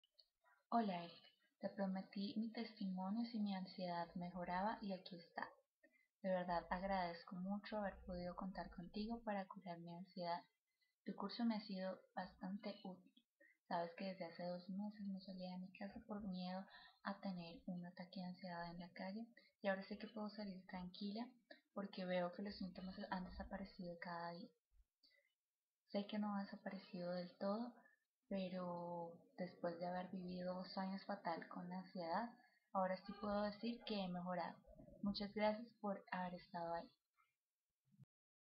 Como he podido curar mi ansiedad : ViveSinAnsiedad.com
Este es un testimonio de una de las personas que han realizado el programa de ViveSinAnsiedad.com
Después de tan solo unas semanas, ha podido combatir su ansiedad muchísimo y por fin ha conseguido salir a la calle y poder hacer la vida que tanto echaba de menos.